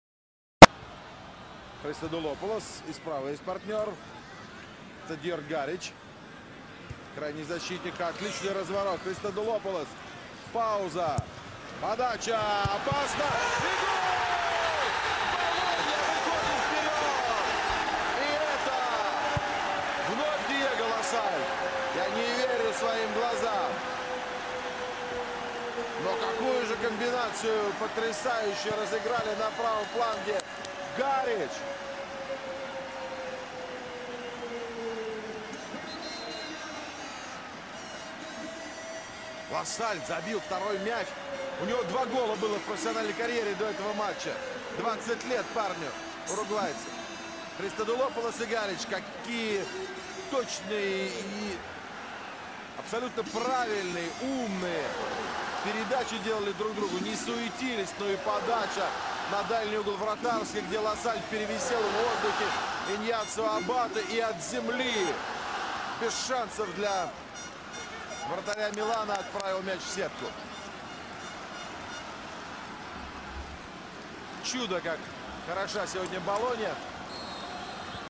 Diego Laxalt The Future of FC Inter at Bologna (2 Goals vs AC Milan) Bologna-AC Milan 2013
Inter Derby this Weekend
Forza Inter !